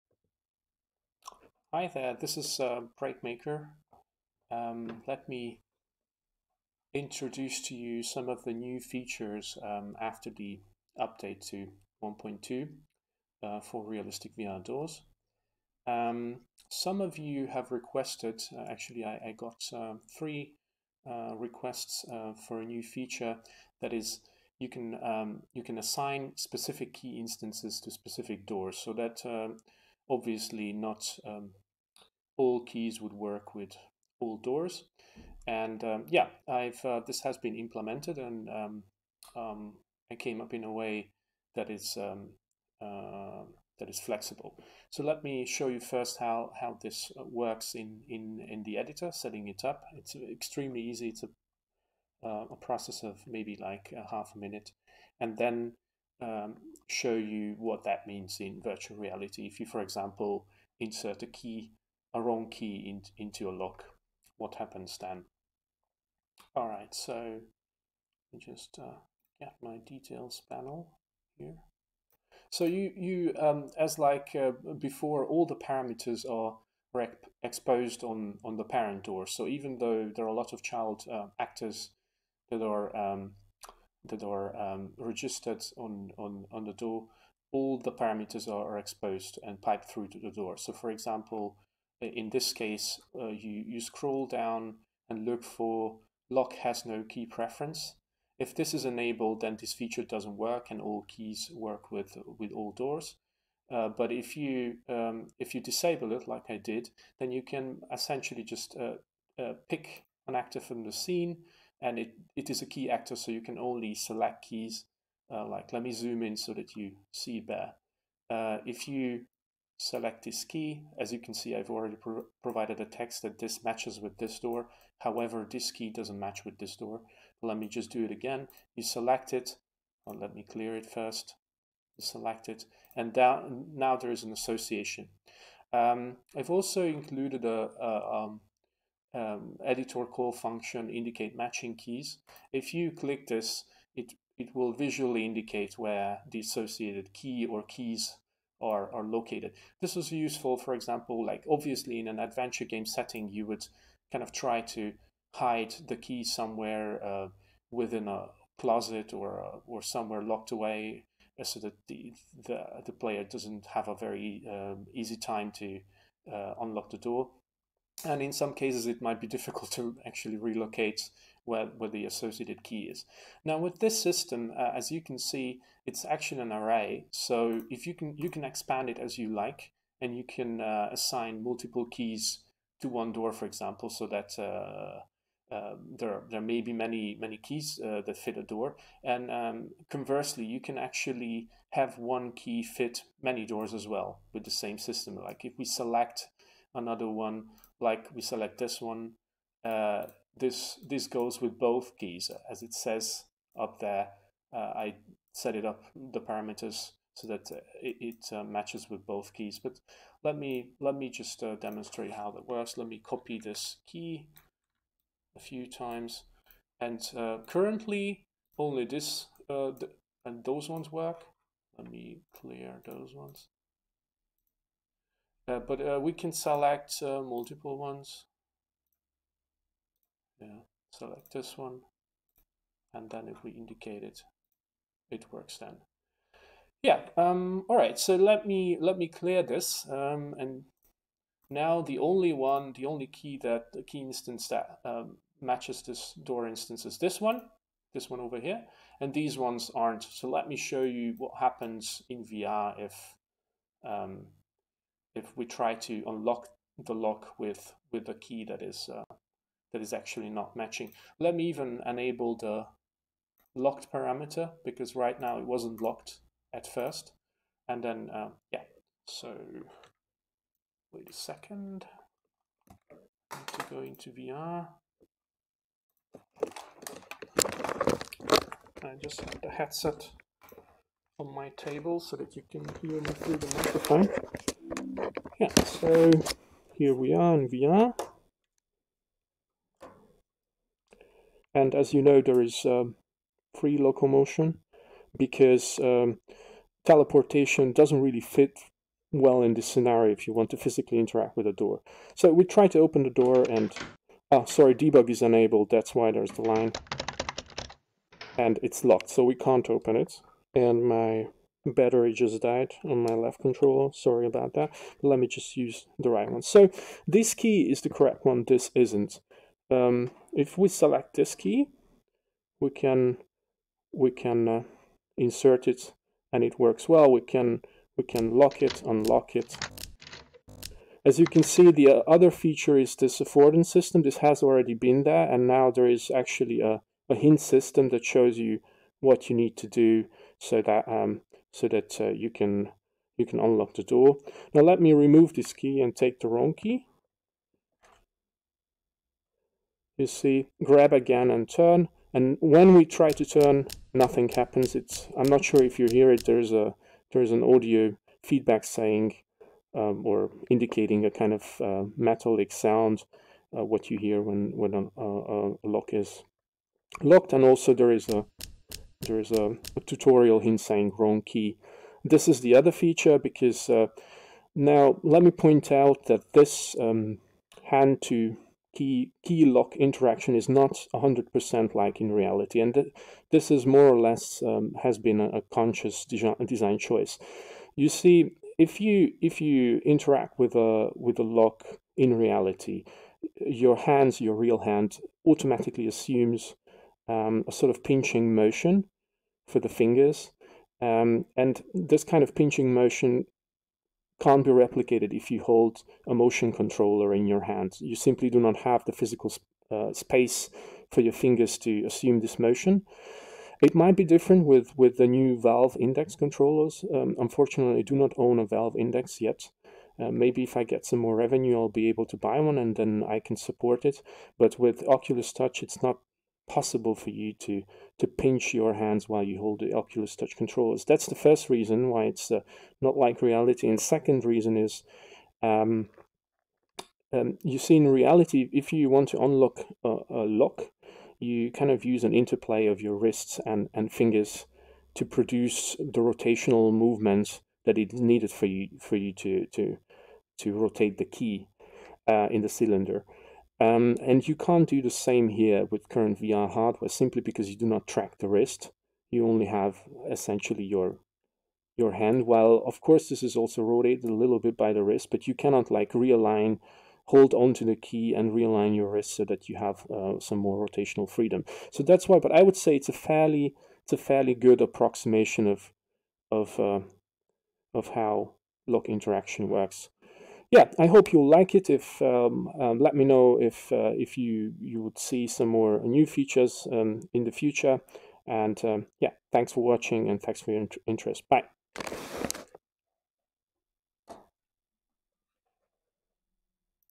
Realistic Virtual Reality Doors - New features with update 1.2
Please check out the two most important developments in regard to Realistic VR doors:

1. It is now possible to assign specific keys to specific doors. The wrong, while still can be inserted into the lock, cannot rotate in the lock cylinder.

2. Tutorial hints can be enabled if needed, which prompt new users what needs to be done to unlock the door.

Interested? Get it on the Unreal Engine 4 Marketplace